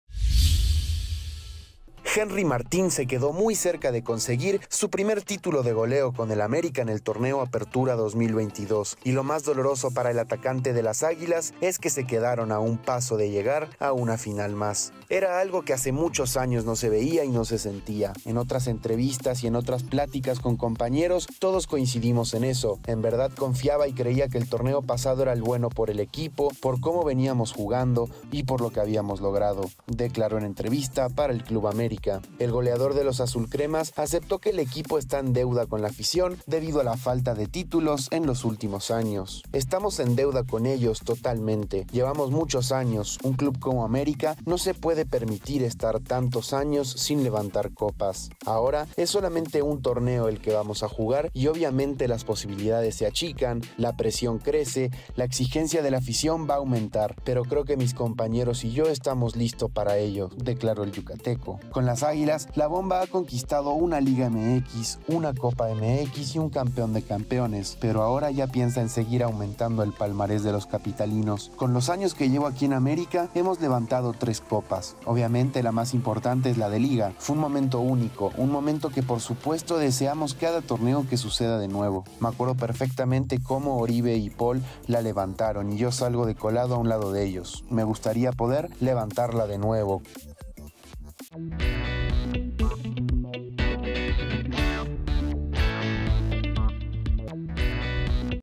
Henry Martín afirma que en un club como América no pueden pasar tantos años sin títulos
El centrodelantero mexicano acepta estar en deuda con la afición de las Águilas por no levantar copas recientemente